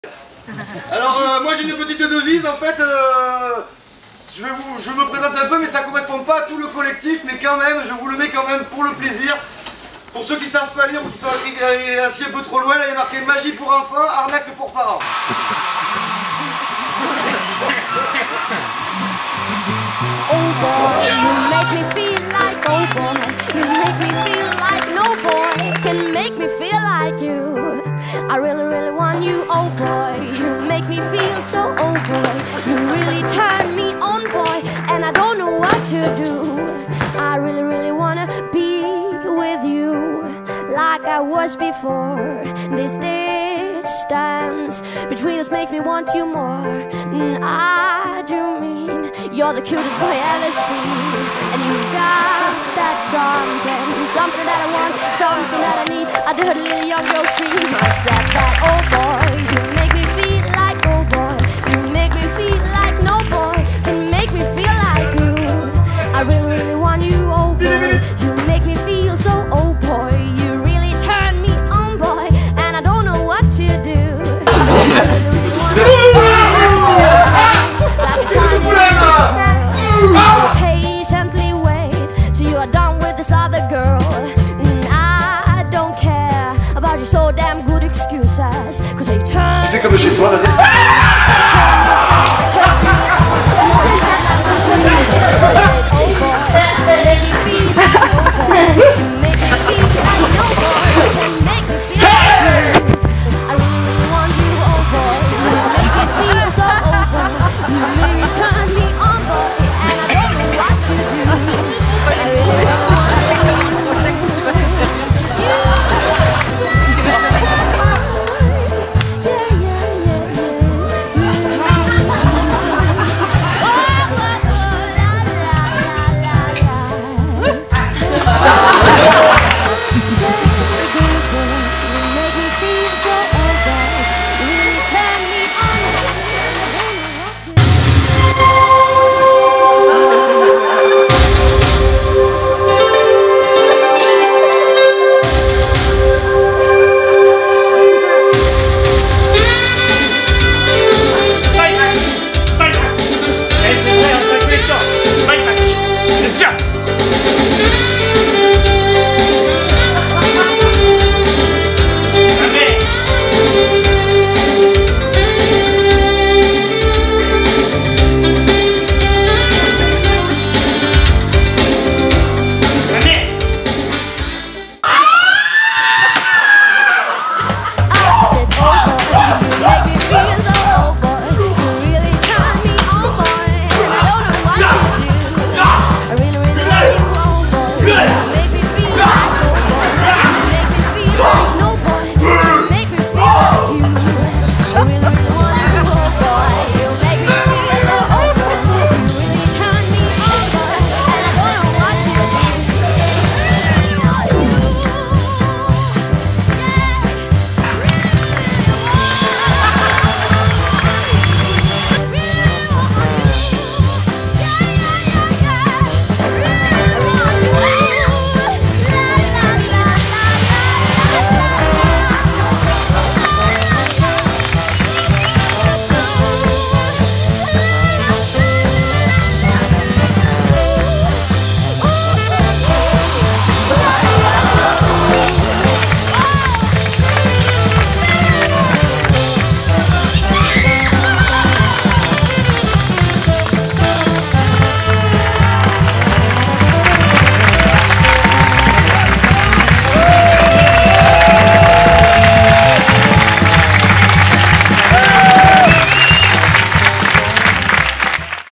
cabaret magie new.mov
This is a trailer from a Magic cabaret performed at Garage Lézart in Lestiac by the collectif Faux-Dépôt. The pieces presented were the result of a week of residency. Featuring: Thierry Forges, Yohan Gaultier, Rémi Lavesnne, Magali Bancel, Julien Mandier, Antoine Terrieux, Stephane Pellicia, Jonathan, Mathieu Moustache et Bertrand.